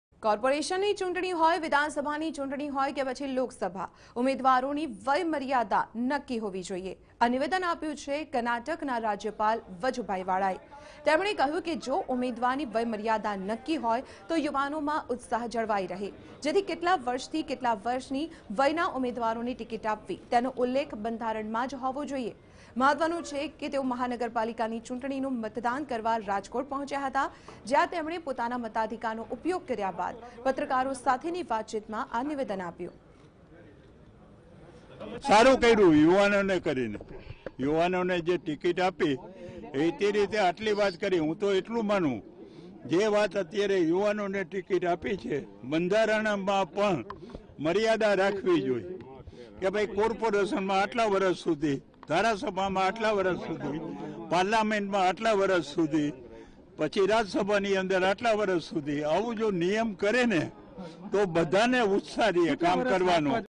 Age limit of election candidates should be mentioned in constitution: Karnataka Governor | TV9News
Age limit of election candidates should be mentioned in constitution: Karnataka Governor | N15